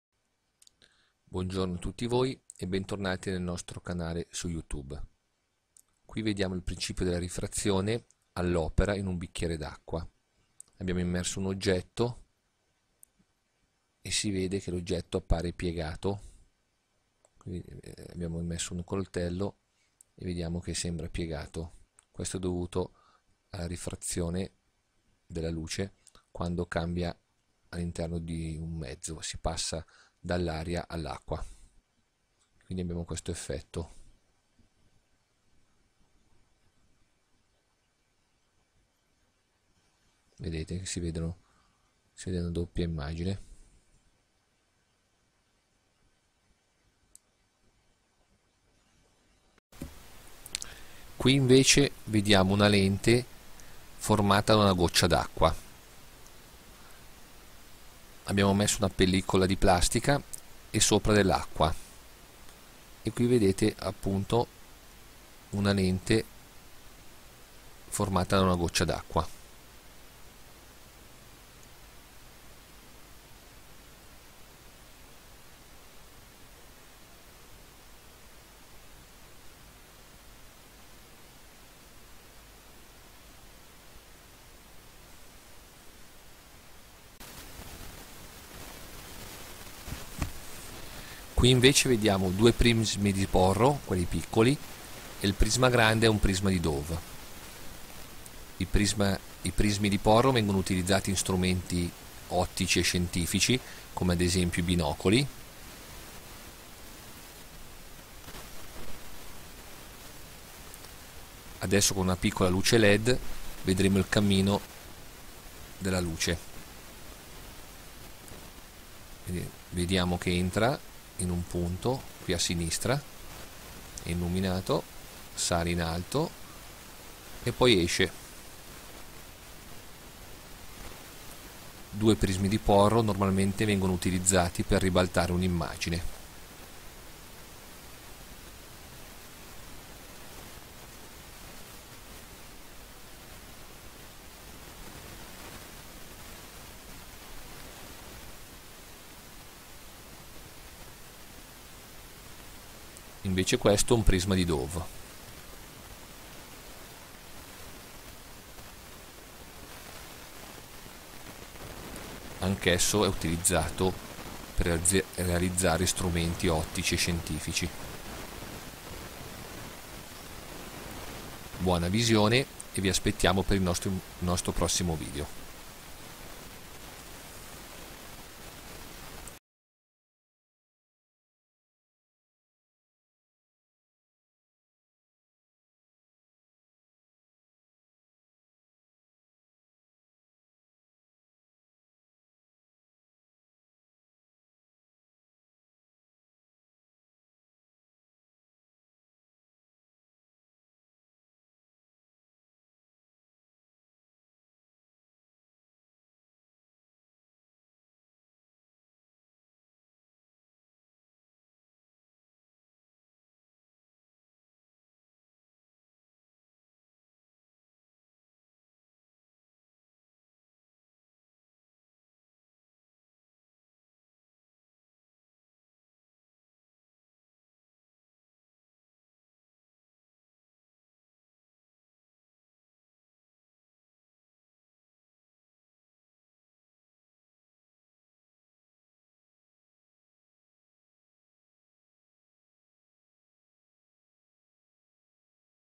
Ottica per tutti. Impariamo l'ottica in modo pratico e utile. Ottica geometrica per tutti.
Piccoli esperimenti in ottica e alcuni concetti pratici. Rifrazione, lente con una goccia d'acqua, prismi di Porro e prisma Dove.